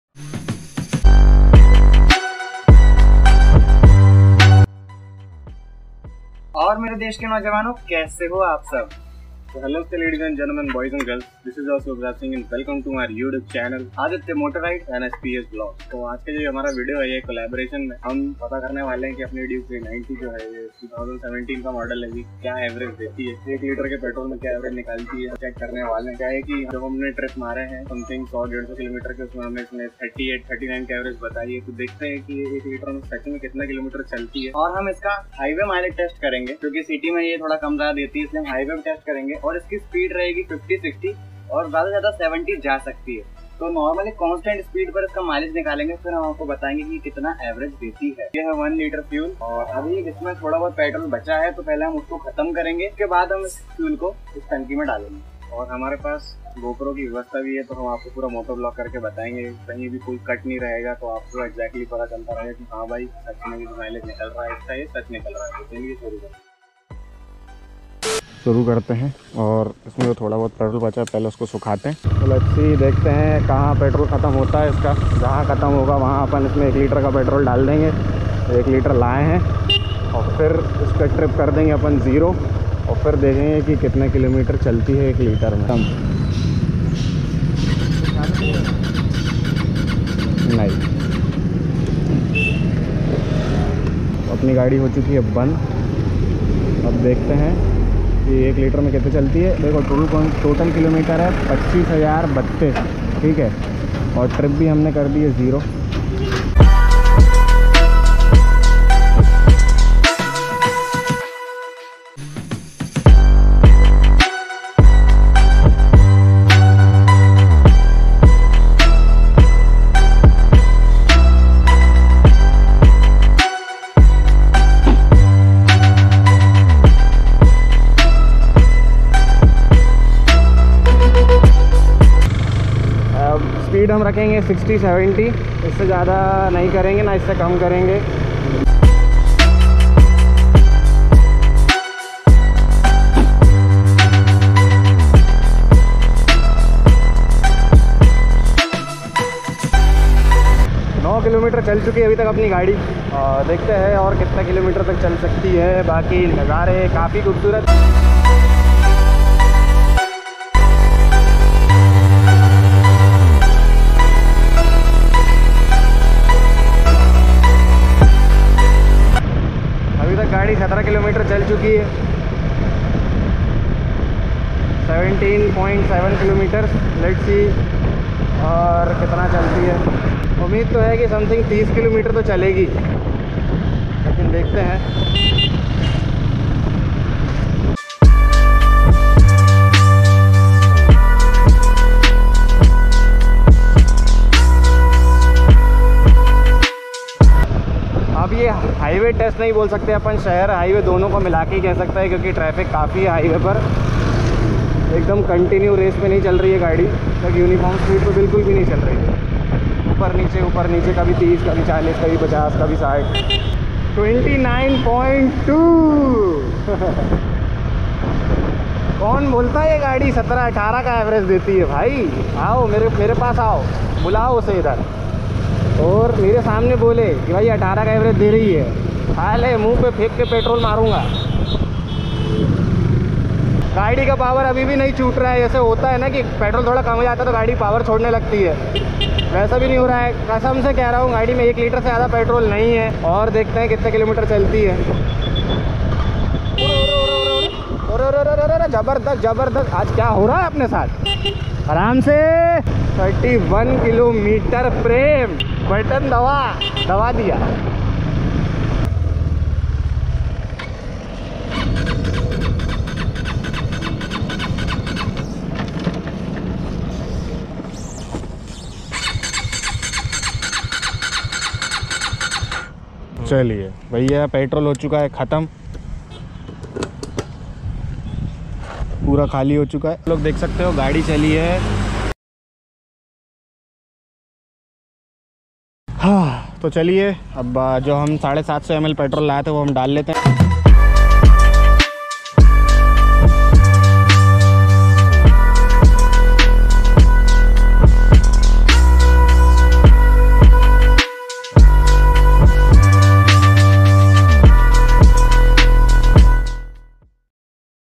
Real Mileage Test ktm duke 390 after 25000 Km||1litre||Better then bs6 duke 390??||Must watch.
In this video we describe (Real Mileage Test of KTM Duke 390 bs4 or me yeh bhi bol sakta hun ki iska mileage bs6 duke 390 se bhi accha ha 👍)...friends this bike is most loveable to all teenagers in this generation.

And also we have done collaboration with Sps vlogs so do check it out his YouTube channel for interesting vlogs.